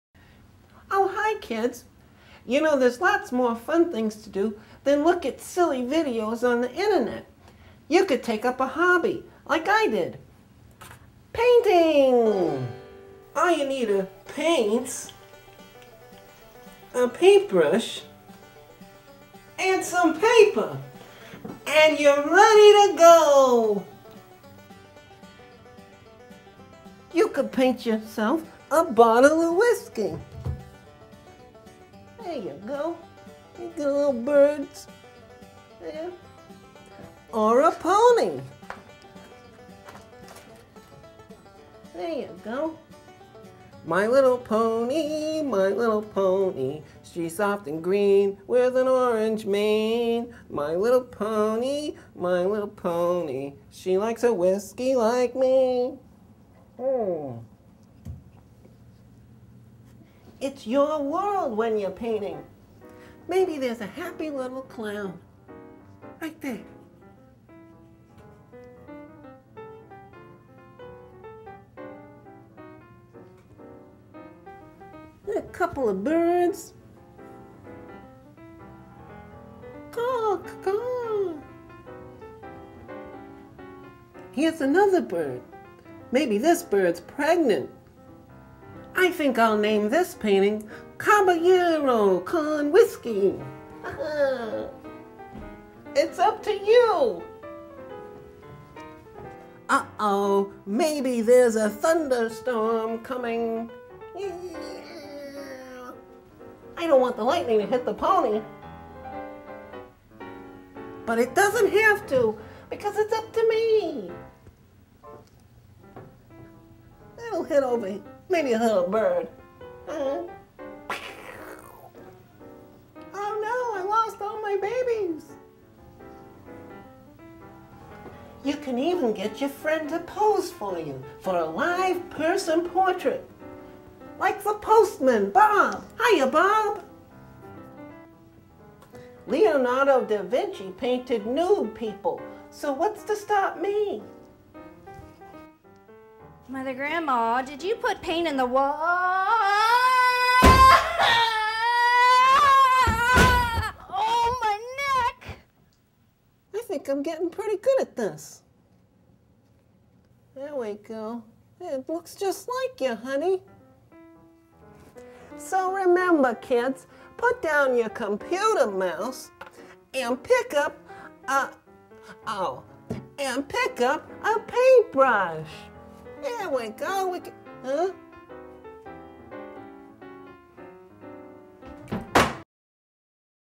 Mother Grandma Painting the Pony
Mother Grandma has found a new hobby...
with Barry McLaughlin as Bob the Postman